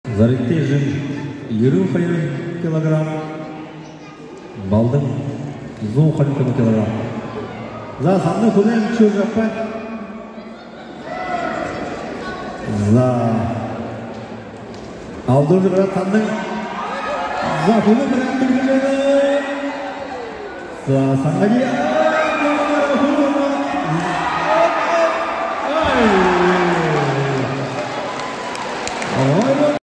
Бурятская борьба «Бүхэ барилдаан». Финал 75 кг. Сангади Ульзетуев- Сандан Гармаев
27 января 2024 г. Республиканский турнир по бурятской борьбе «Бүхэ барилдаан», посвящённый Хамбо ламам Гоман дацана Галсан Лыгден Аржагарову и Агван Нима Цыдыпдоржиеву среди мужчин и юношей младше 2007 года рождения.